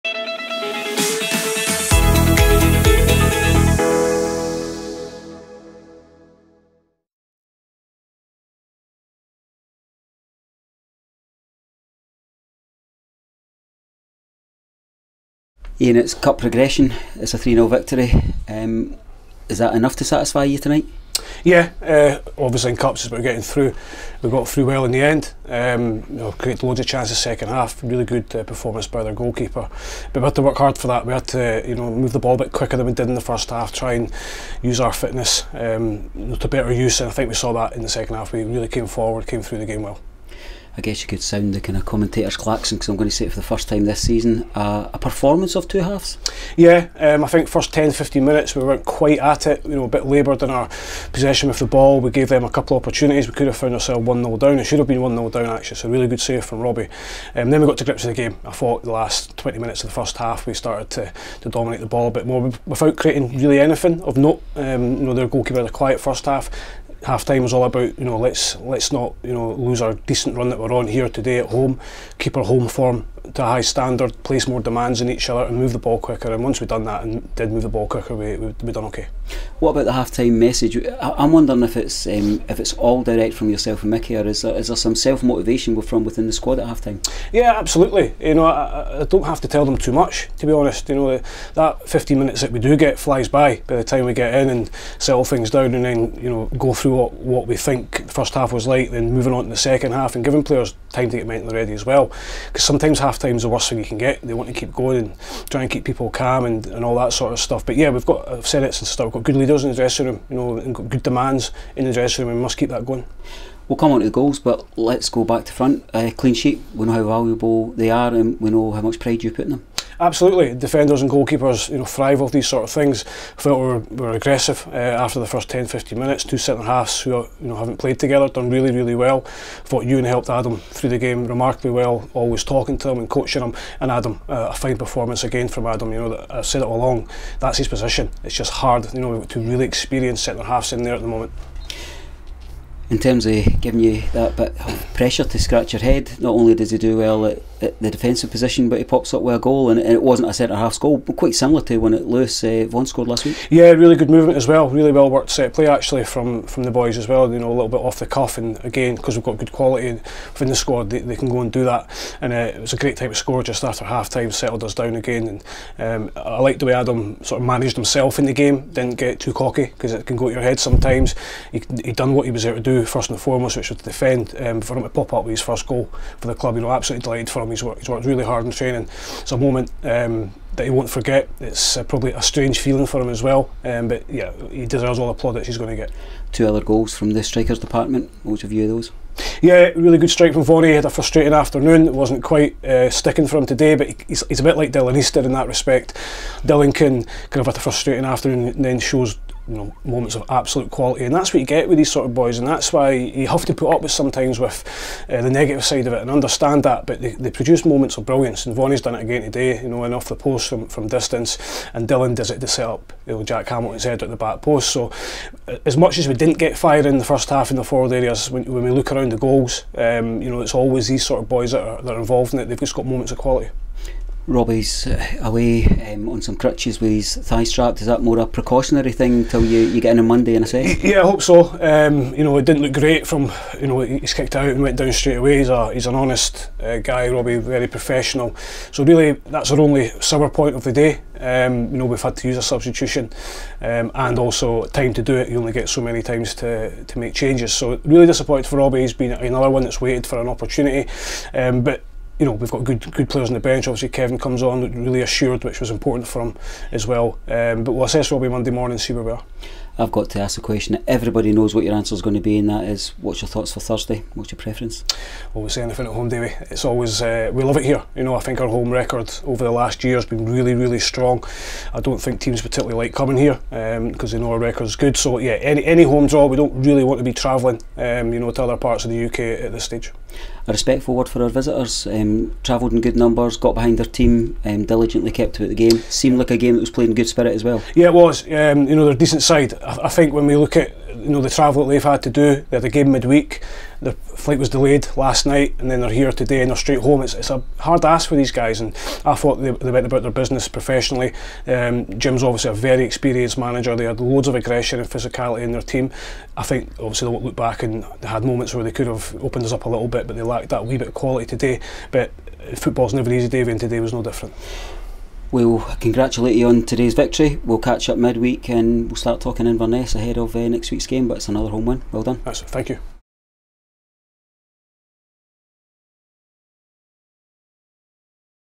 Cliftonville Post-Match with Ian Murray
Manager Ian Murray spoke after the SPFL Trust Trophy win over Cliftonville.